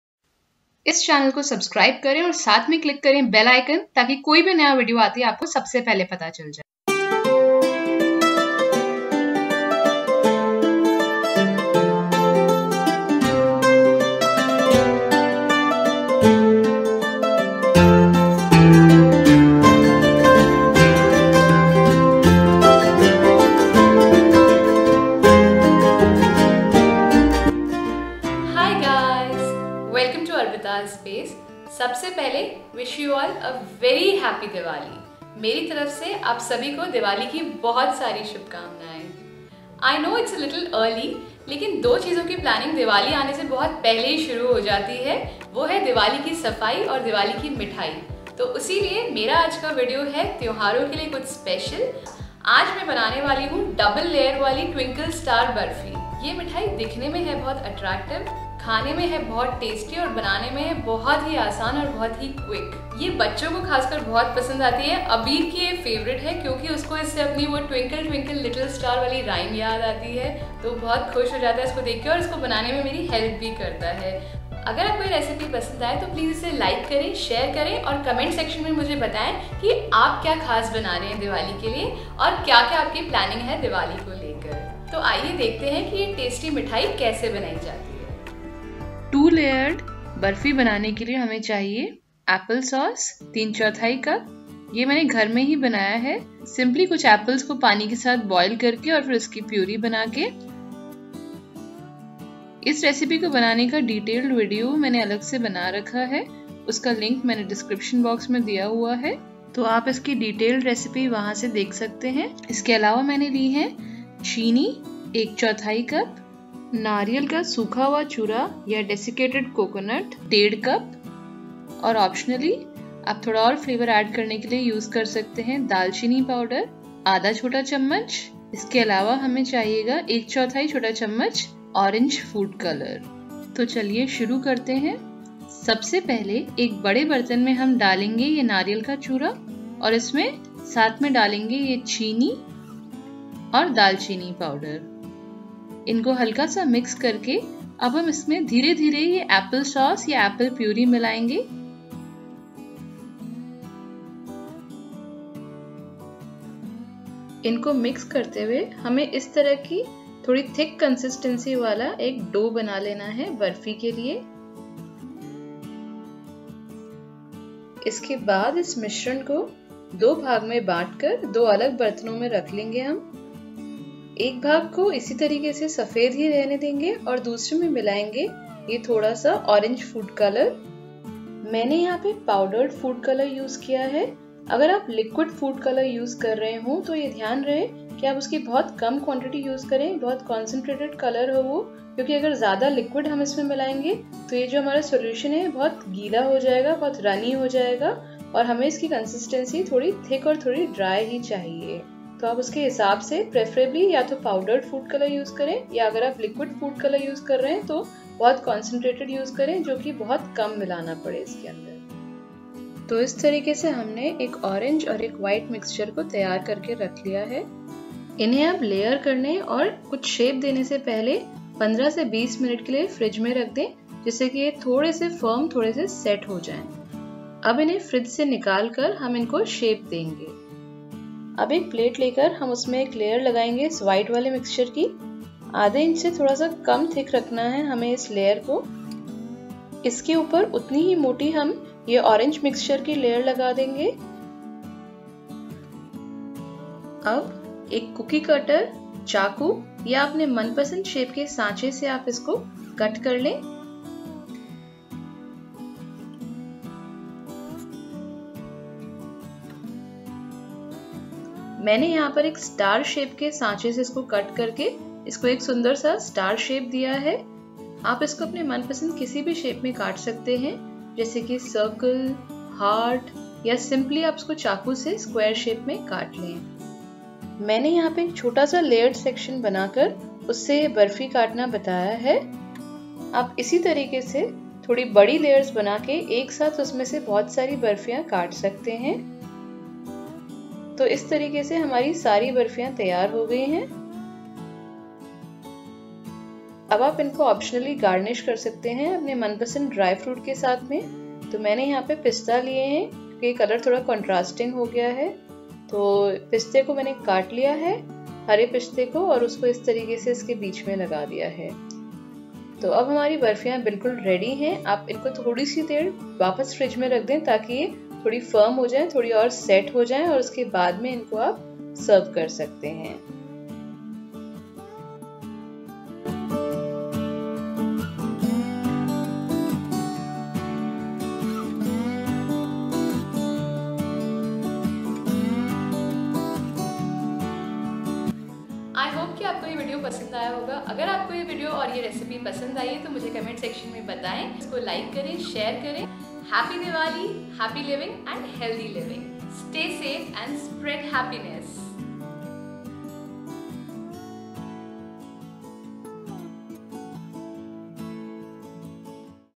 Instant Fudge|Double layer Star barfi|मिनटों में बनाएं मार्किट जैसी टेस्टी मिठाई|Arpita's Space
In this video, learn how to make Diwali or any occasion special Mithai/sweet/fudge/Burfi. It is a 3 ingredient No cook recipe which requires just 5 minutes to be made(provided the ingredients are ready to use).
It looks like it has been bought from the market/sweets shop, since it has a unique star shape and is double layered with 2 different colors.

INGREDIENTS:

Applesauce : 3/4 cup
Sugar : 1/4 cup
Desiccated Coconut : 1.5 cup
Cinnamon Powder : 1/2 tsp
Orange food color: 1/4 tsp
Pistachios(Optional): 5-6

Products used:
Cling Wrap
Ice Cream Mould
Kulfi Mould

Equipments used:

Camera Holder clip